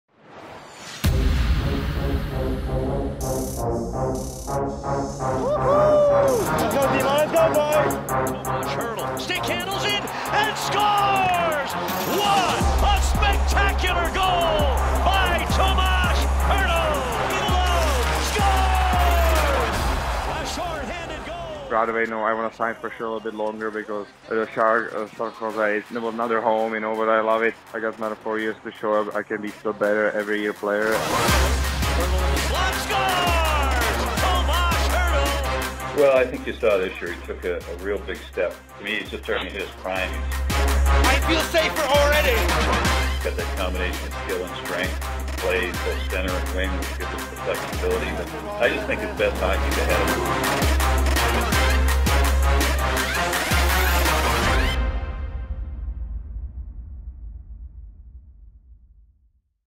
Sharks Re-Sign Tomas Hertl
The San Jose Sharks re-sign forward Tomas Hertl to a four-year contract.